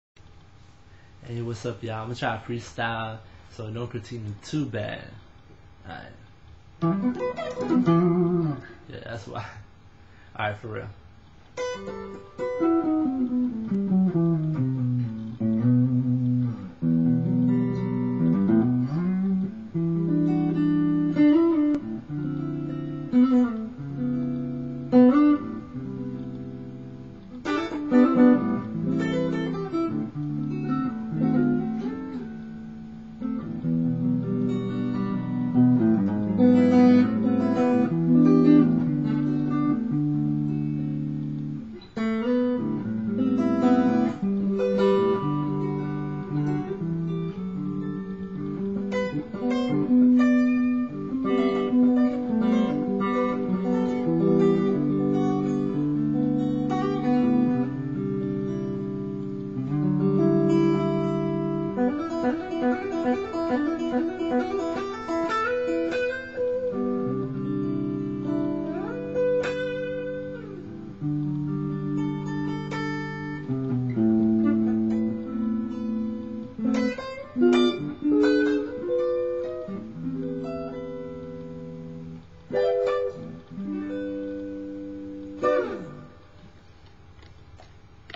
Xeryus Gittens Messing around lol
Im trying to learn these chords people